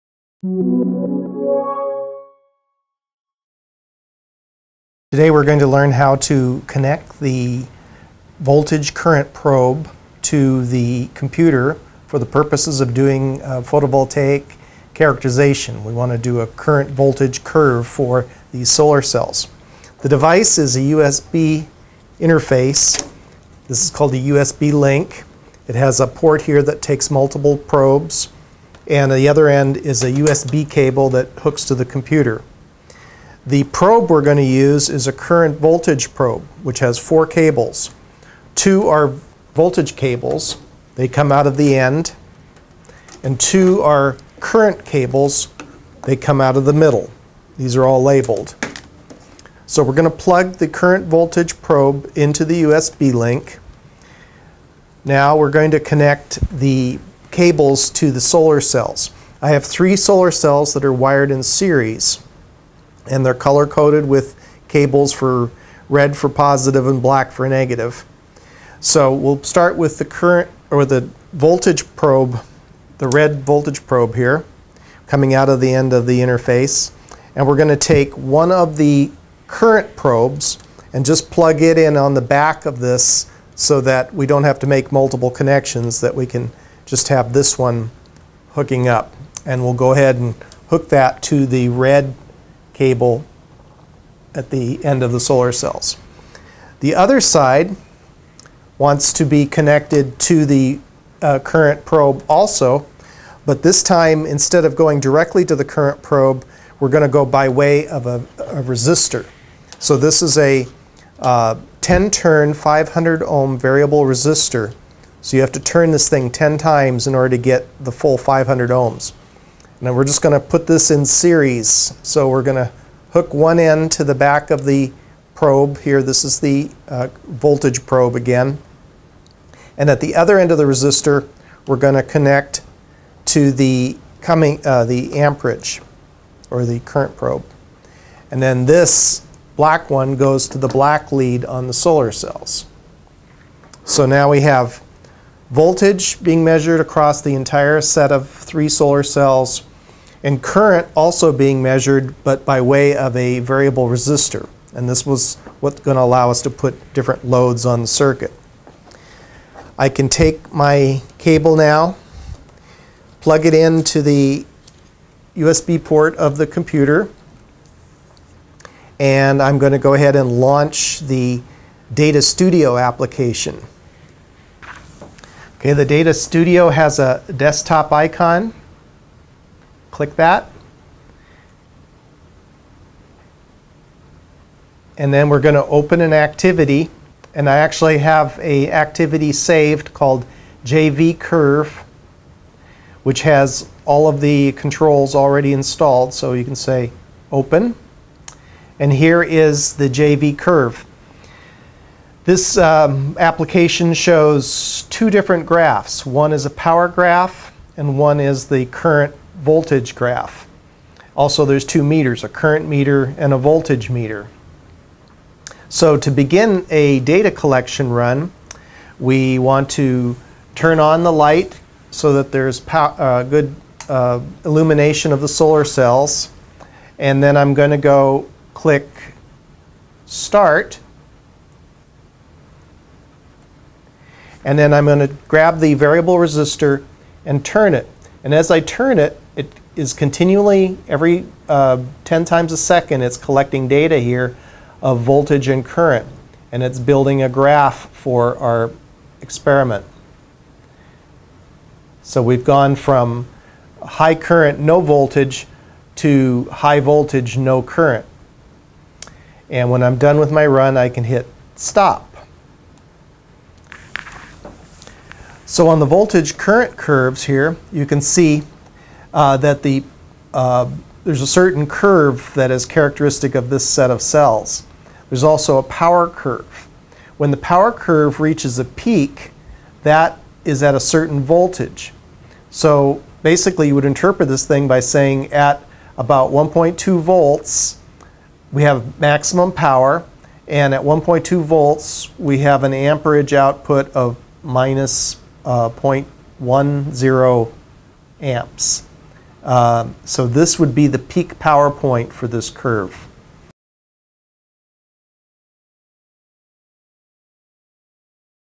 Photovoltaic characterization with probeware
This is a simple lab that can be done in a high school classroom. It produces a current voltage curve which is the only way to accurately measure the performance of solar cell.